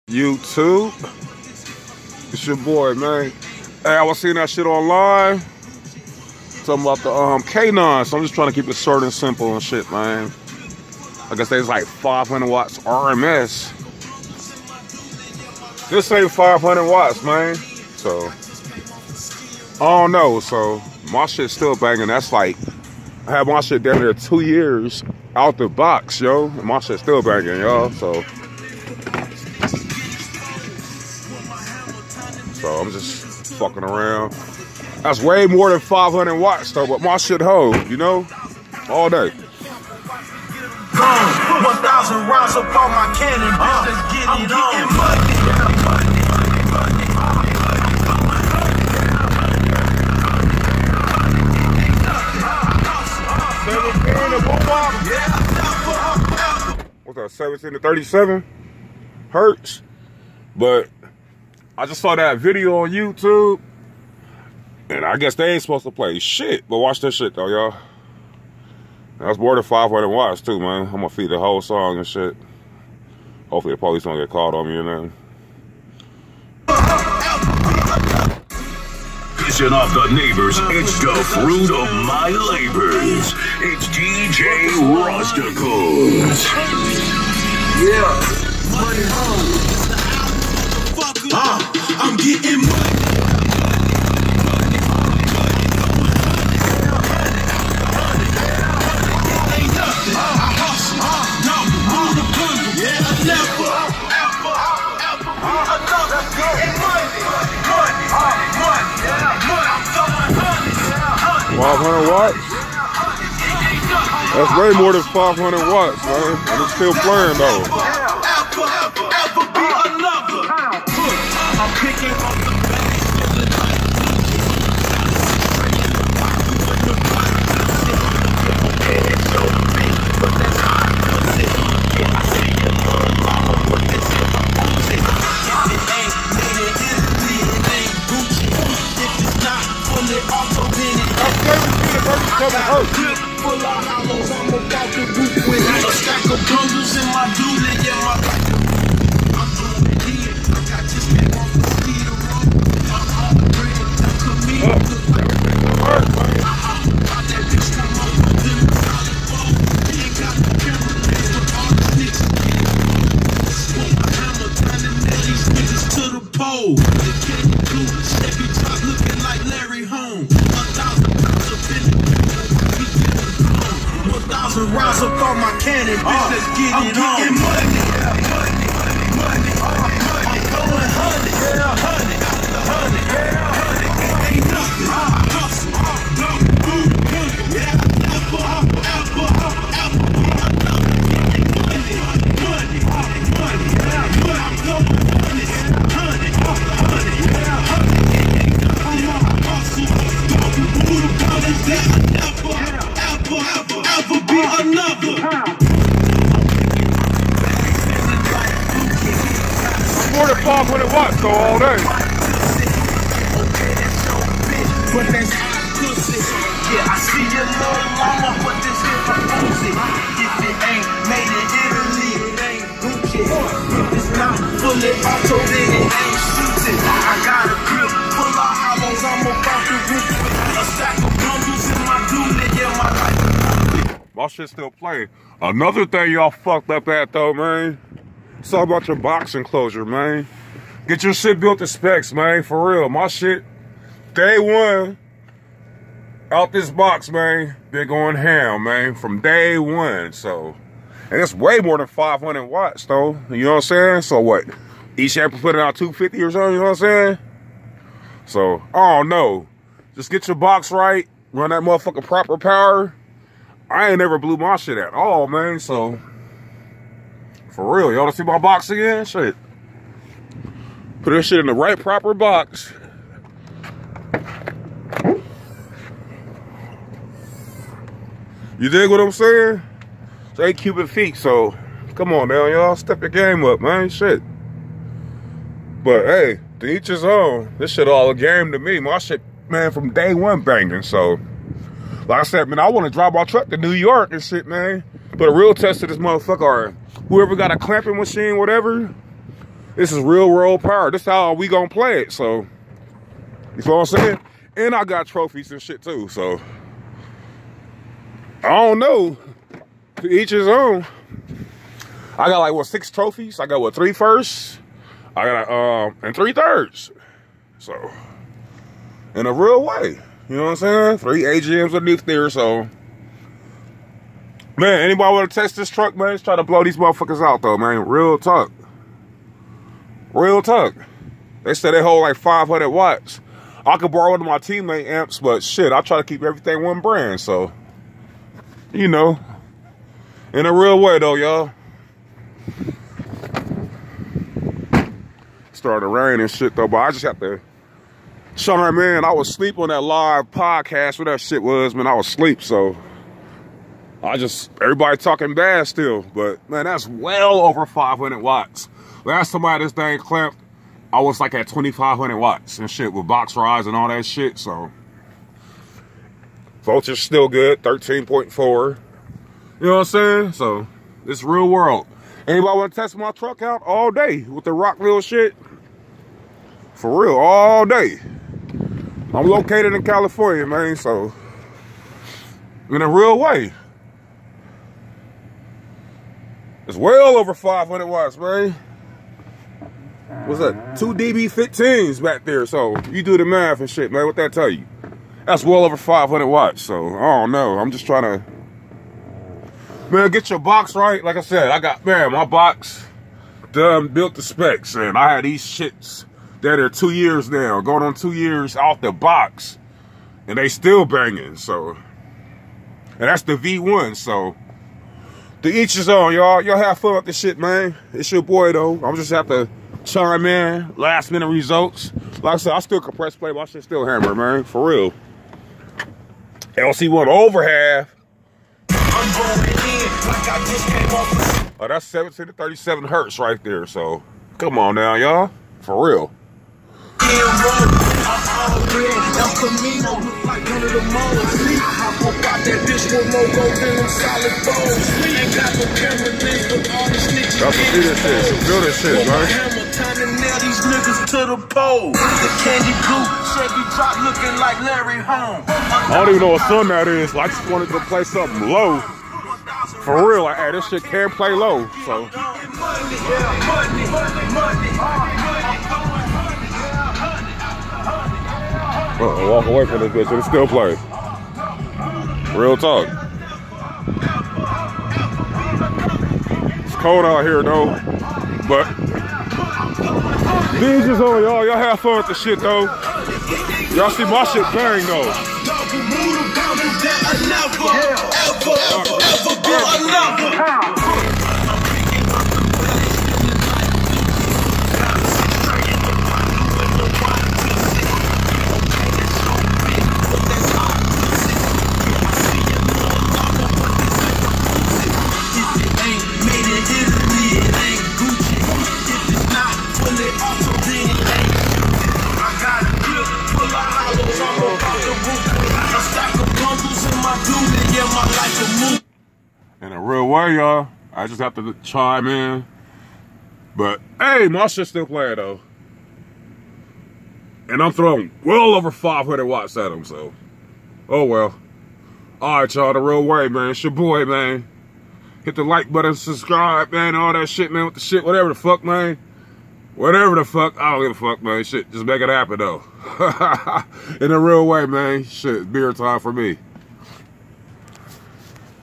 ROCKVILLE AUDIO
K9 v1 taking way over 500 watts... I beat my subs daily for hours. anybody wanna test my rig out.